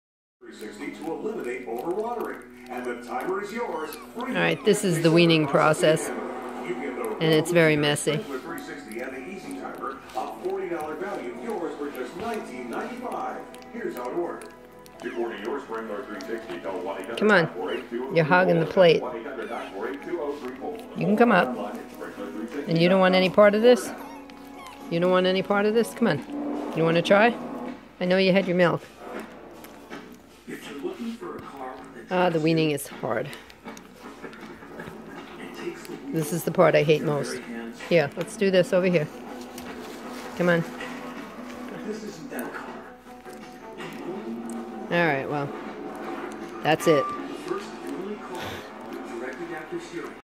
Weaning the puppies
Pekingese puppies at 4.5 weeks first taste of real food